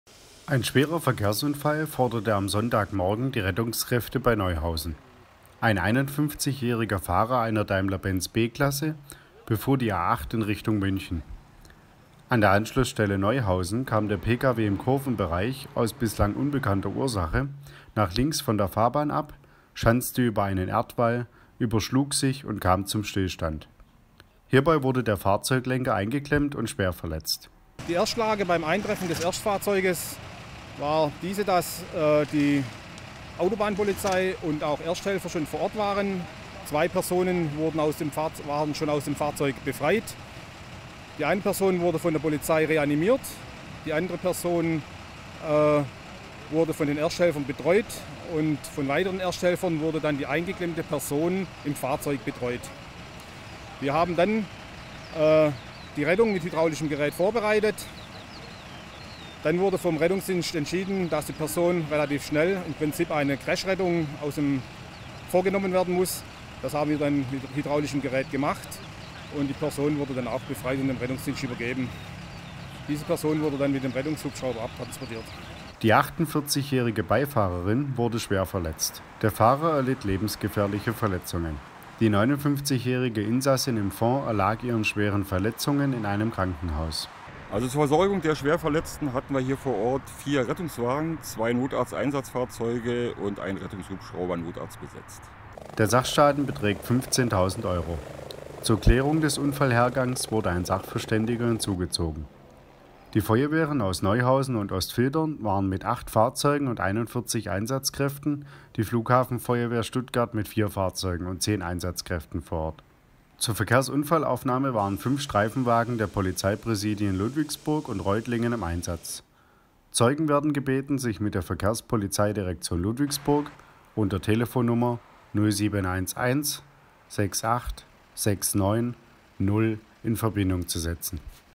22.05.2017 - Schwerer Auffahrunfall fordert Verletzte bei Neuhausen
Neuhausen (red) – Am Montagnachmittag kam es auf der Plieninger Strasse in Neuhausen zu einem folgenschweren Auffahrunfall mit fünf Fahrzeugen. Wie es zu dem Unfall kam ist noch nicht bekannt. Eine Person wurde in ihrem Fahrzeug eingeklemmt und musste von der Feuerwehr Neuhausen, die mit vier Fahrzeugen und 24 Einsatzkräften vor Ort war, befreit werden. Mit schweren Verletzungen kam sie mit einem Rettungshubschrauber in eine Klinik. Die Plieninger Straße wurde während der Unfallaufnahme voll gesperrt. Die Polizei hat die Ermittlungen zum Unfallhergang aufgenommen.